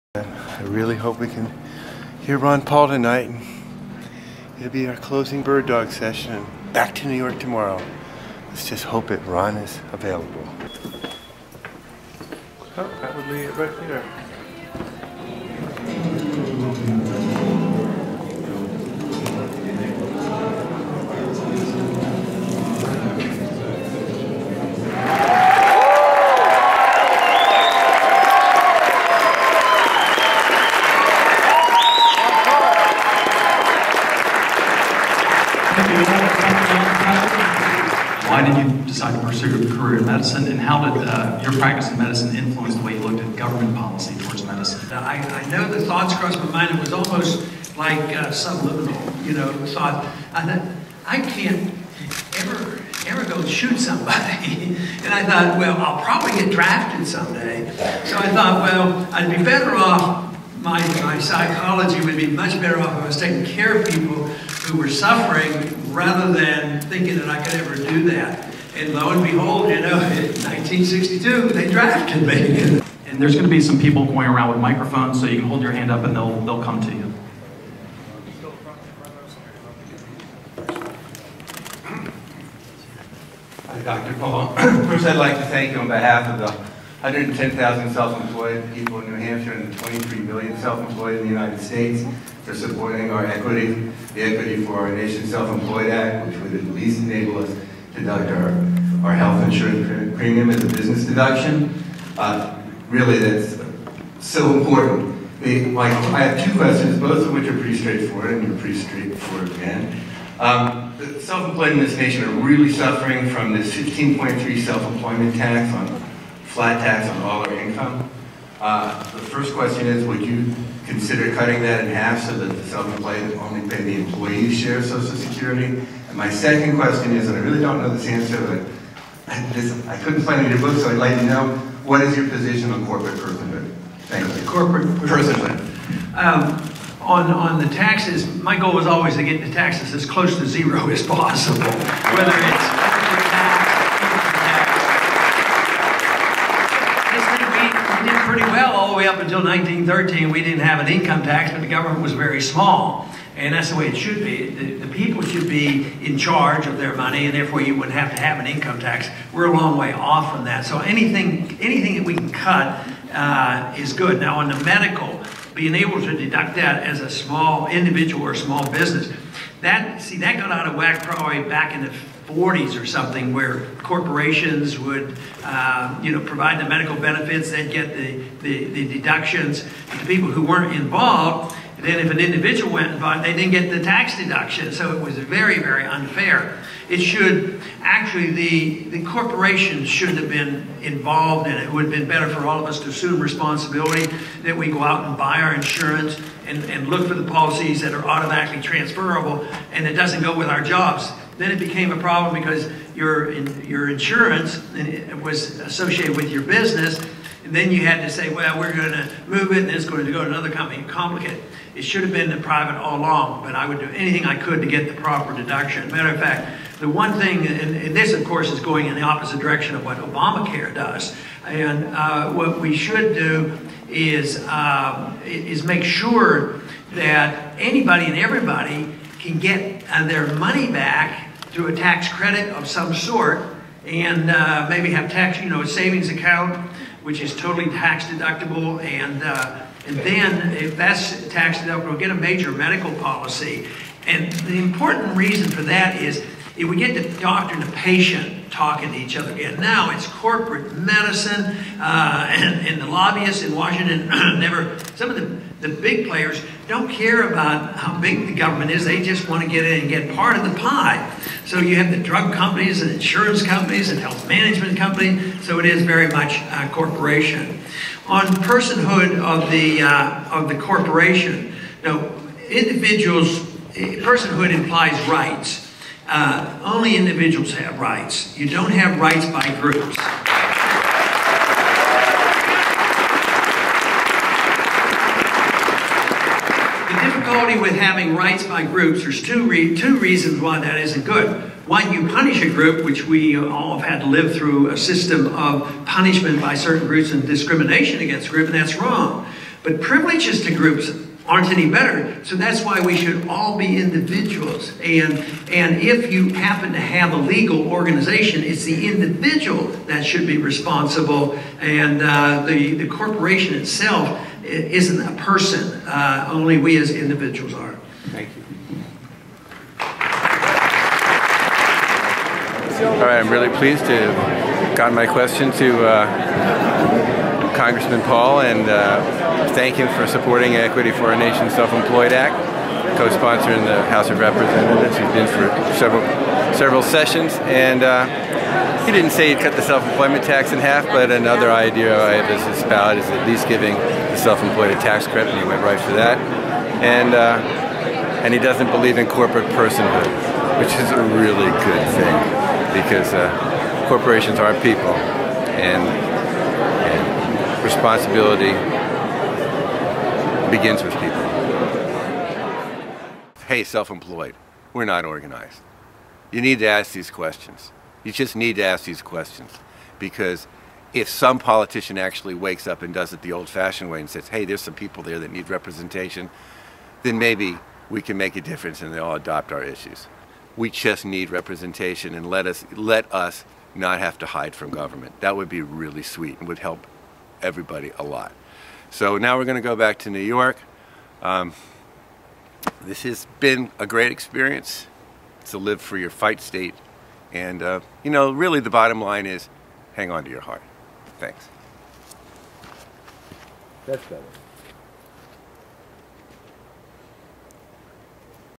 Ron Paul: Corporations are not people
A weary Mark Dunau makes his way to the final stop of this year's birddogging trip. Ron Paul does not make any specific commitments regarding the self-employment tax, but he is refreshingly clear in his answer to the notion of corporate personhood, which he states in terms of individual rights. Then it's time for Mark and cameraman Richard to go home.

January 6th
Durham, New Hampshire
Hang onto your heart.

Recorded by Richard Moran
Edited by Aaron Engelman & Richard Moran